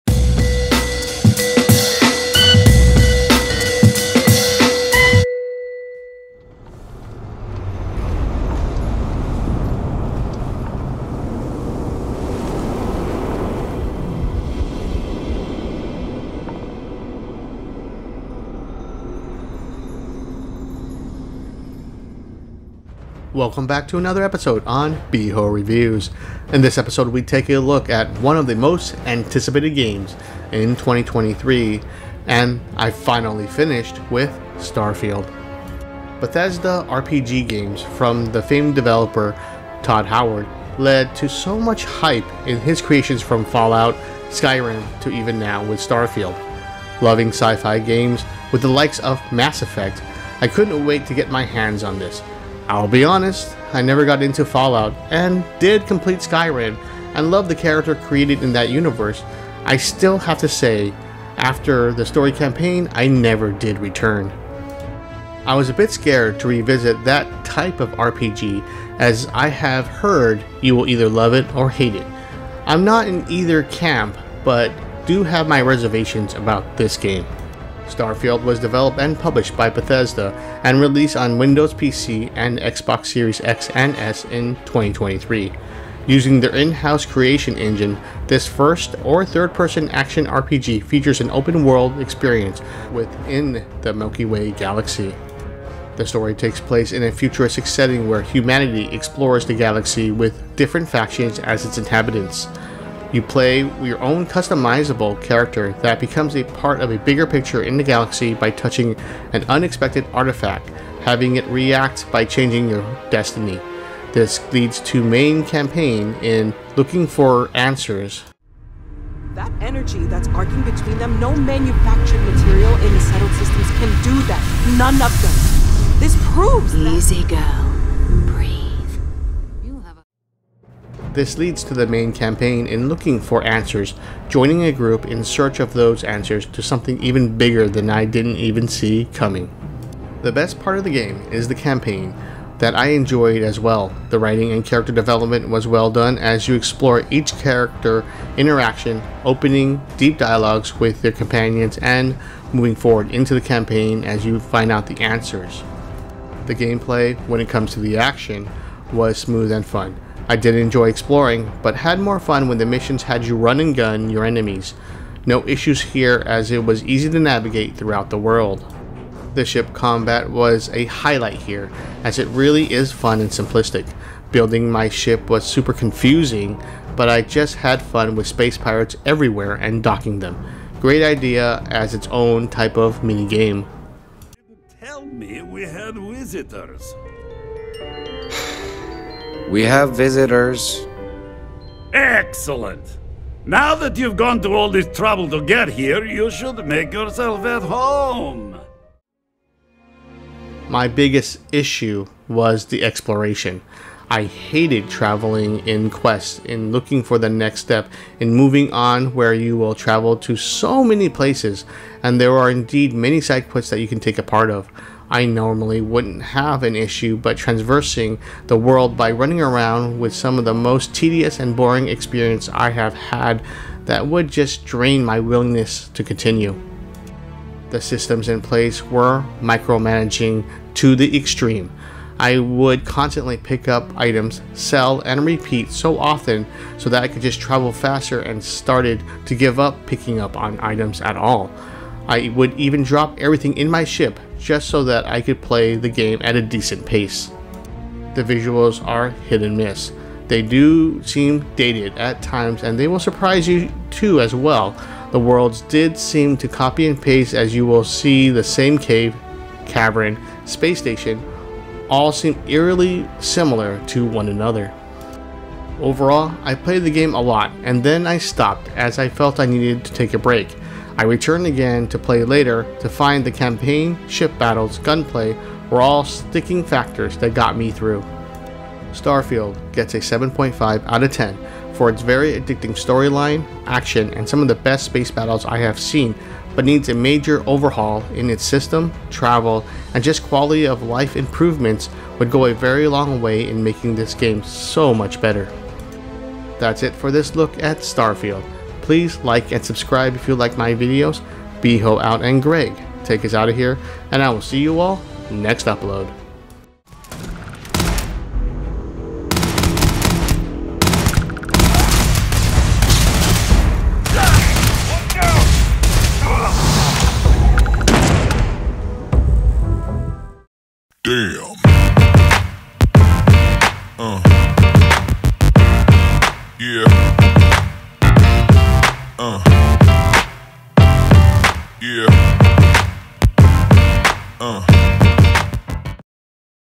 STARFIELD Review
Game Review of highly anticipated game in 2023 with Starfield.  Starfield is a 2023 action role-playing game developed by Bethesda Game Studios and published by Bethesda Softworks. In the game, the player character joins a group of space explorers who must venture to various corners of the galaxy to acquire mysterious artifacts.